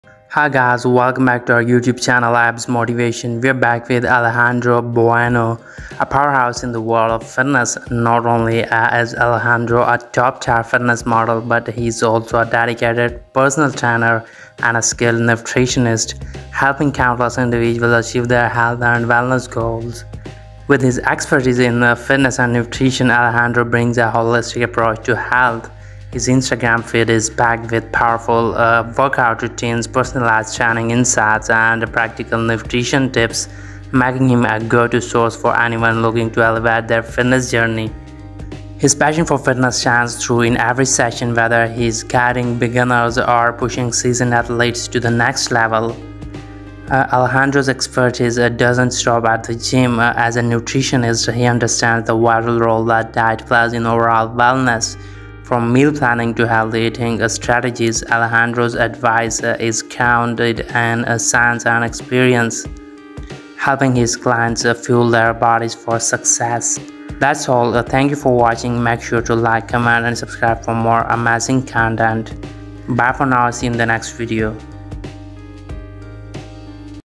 Alejandro Bueno: The Fitness Model Transforming Lives with Training and Nutrition
Video Description

Hi guys, we are back with Alejandro Bueno, a fitness model, personal trainer, and nutritionist dedicated to helping people achieve their health and fitness goals. With a holistic approach that combines expert training techniques and tailored nutrition advice, Alejandro has become a trusted figure in the fitness industry.